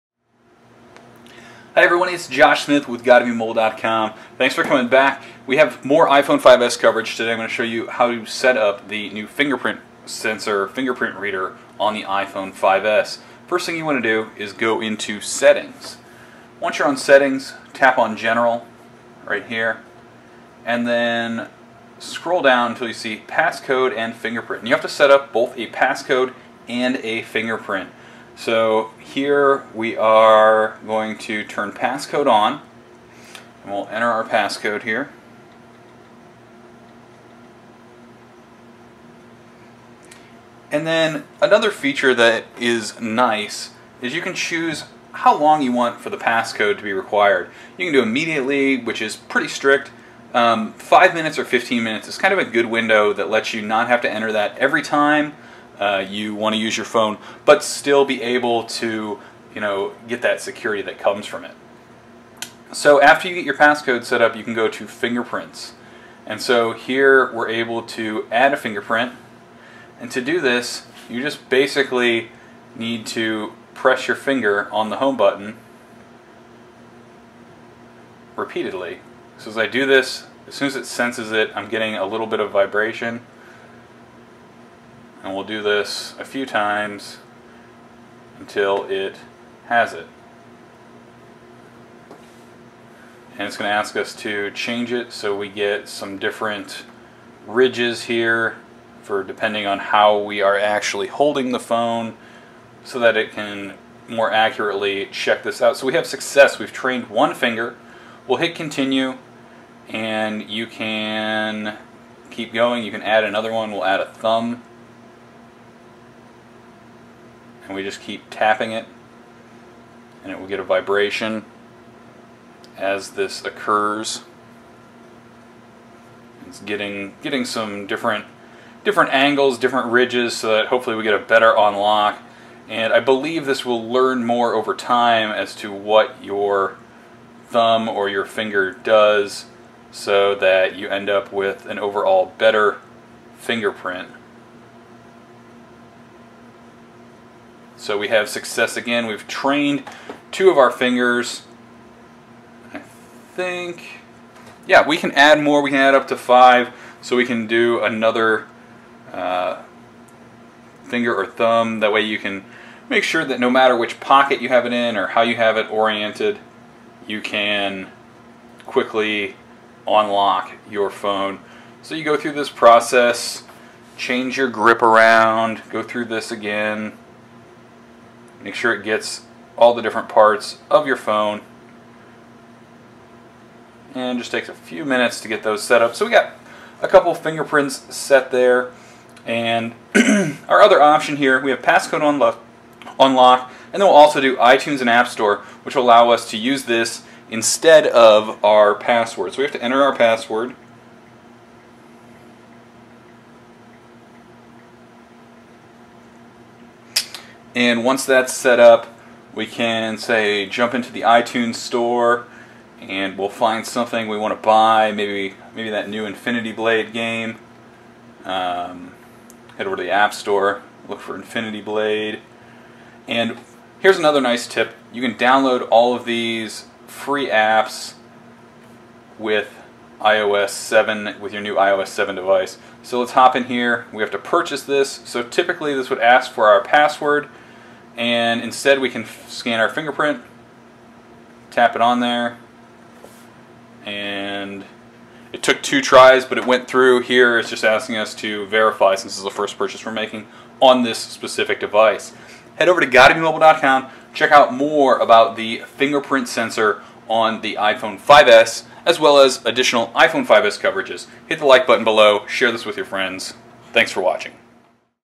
How to Set Up Touch ID on the iPhone or iPad
How to use the Touch ID fingerprint sensor on the iPhone from setup to unlocking and making purchases in iTunes.